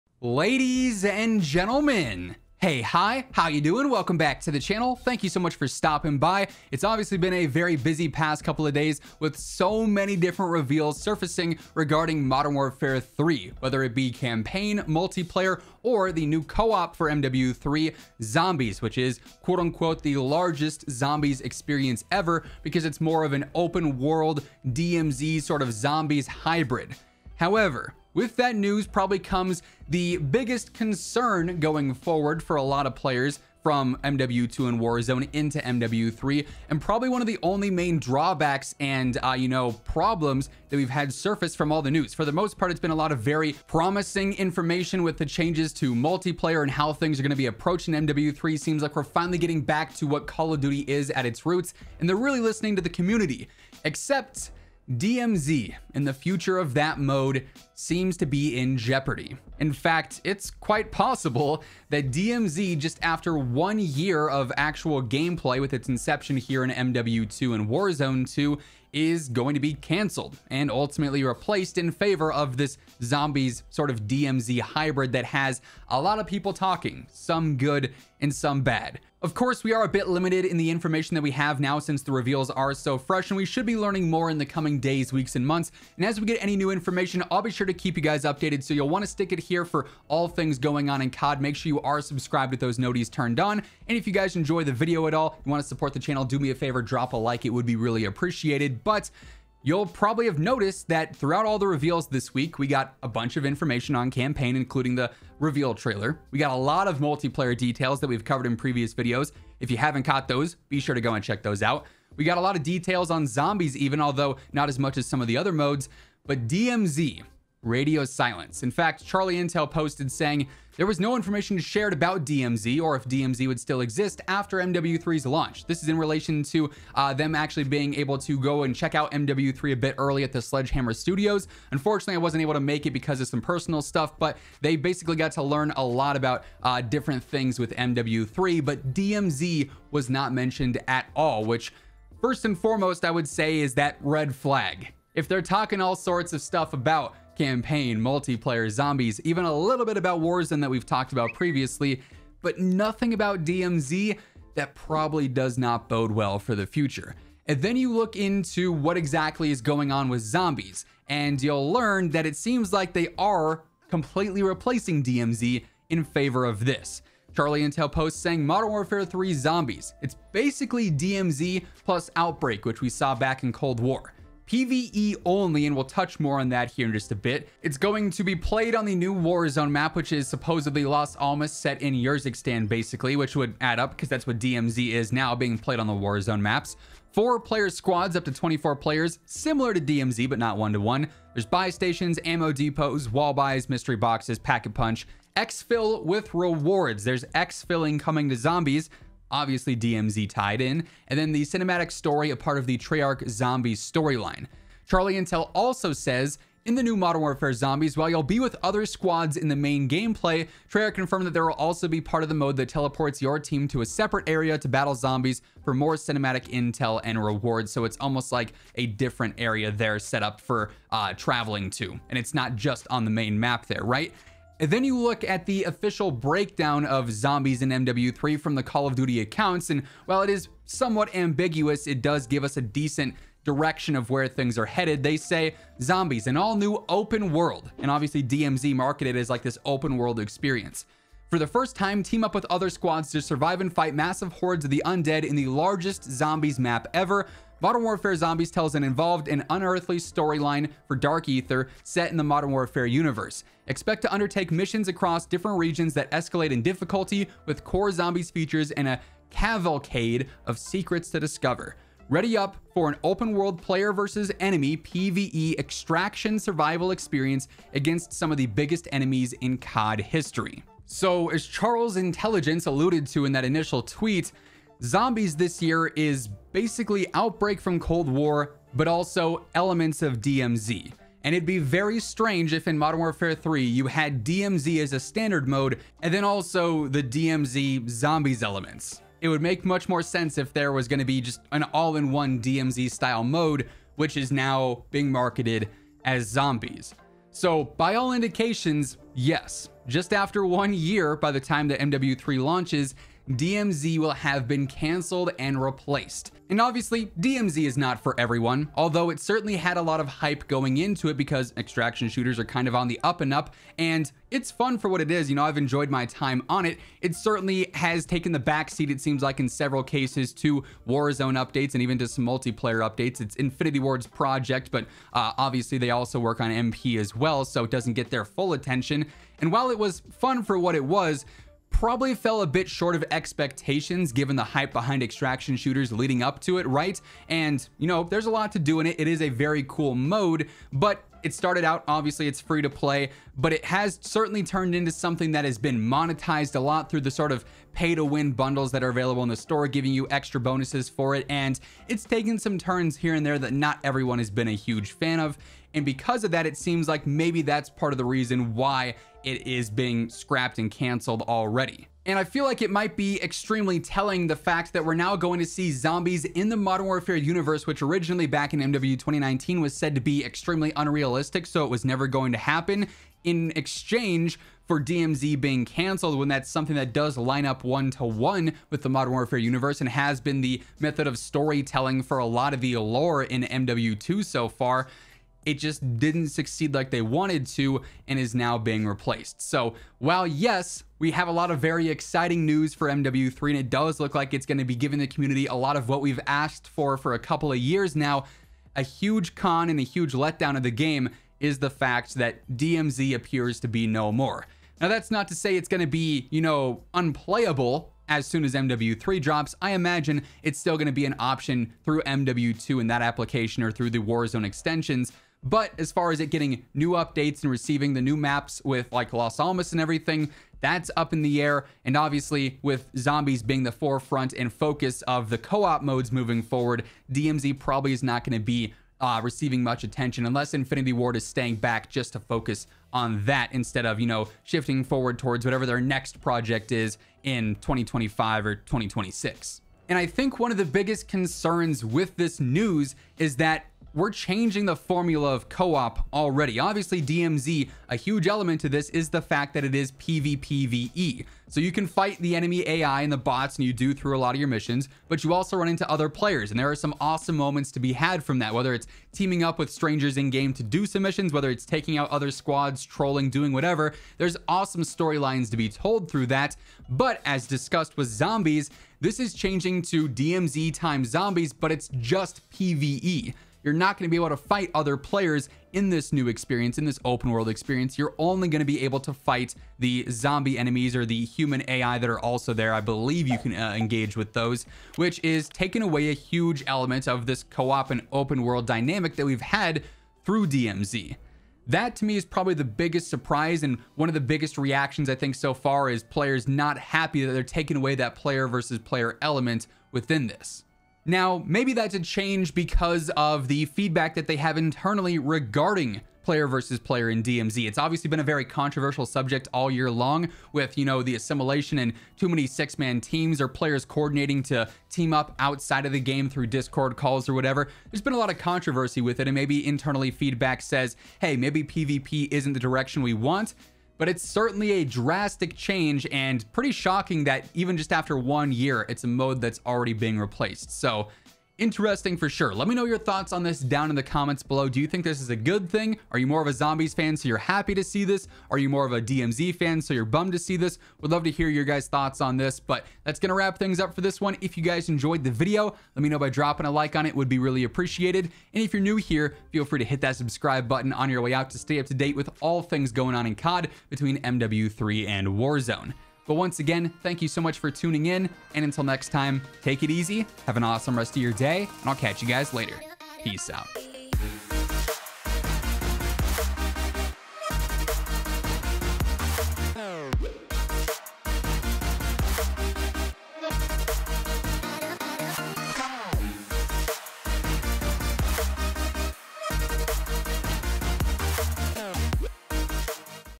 Call Of Duty Just CANCELLED This...
After just 1 year, Call of Duty has seemingly cancelled DMZ...

In this video, we're explaining the new DMZ Update for Modern Warfare 3 and WARZONE! With the recent Modern Warfare III Reveals and announcements, it appears that Call of Duty is Cancelling DMZ moving forward! There currently has been no mention of DMZ is Modern Warfare 3, instead COD is opting for MW3 Zombies as a replacement!